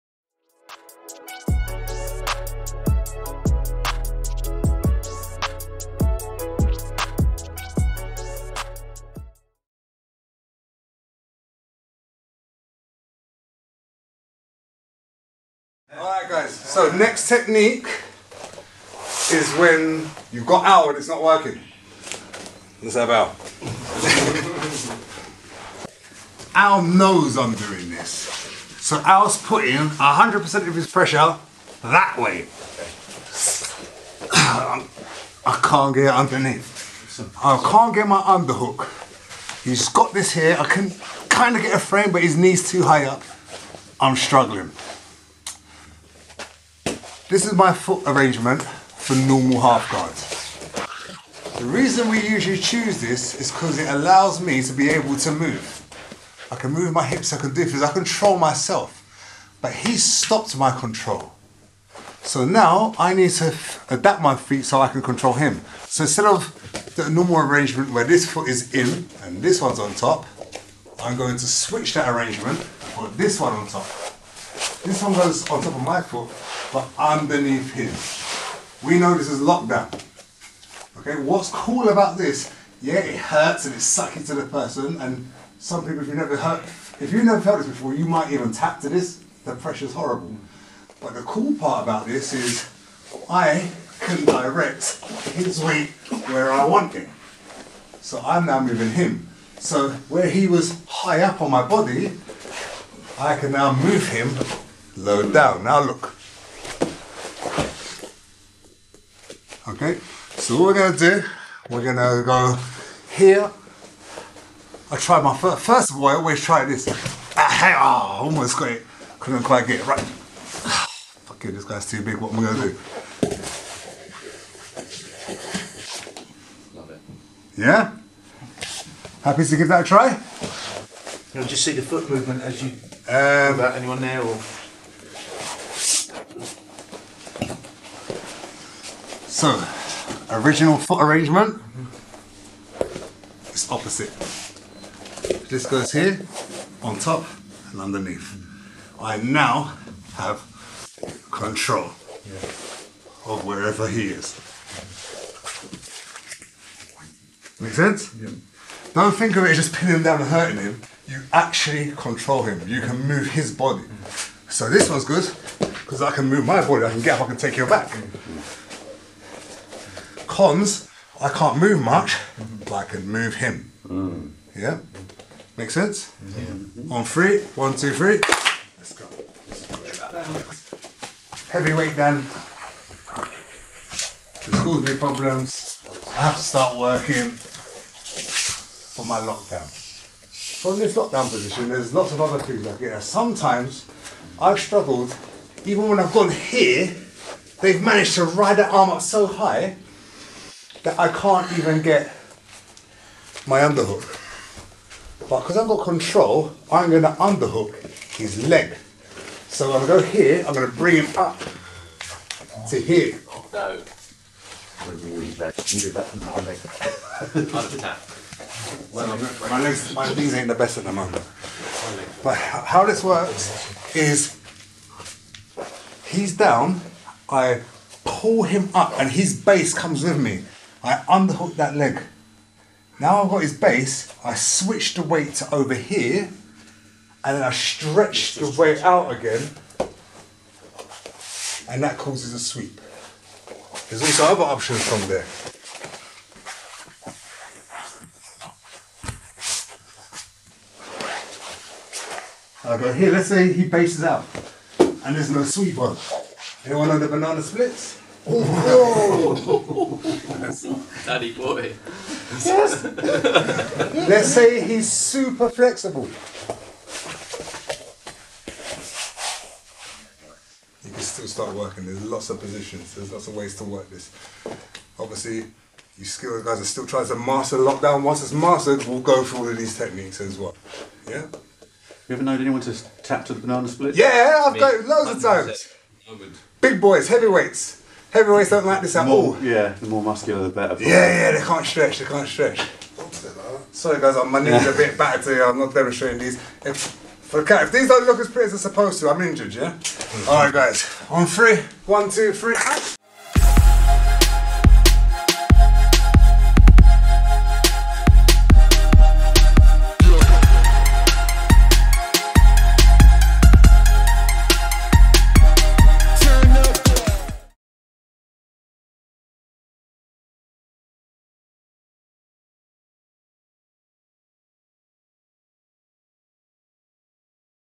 Half Guard Lockdown & Sweep - BJJ No Gi Techniques
Welcome back to the channel! This is a new video with Coach Ryan Robinson demonstrating Half Guard Lockdown & Sweep. In association with MMA Armary.

Please be aware that the actions demonstrated in this video are performed by professionals and should not be tried at home unless supervised by trained professionals.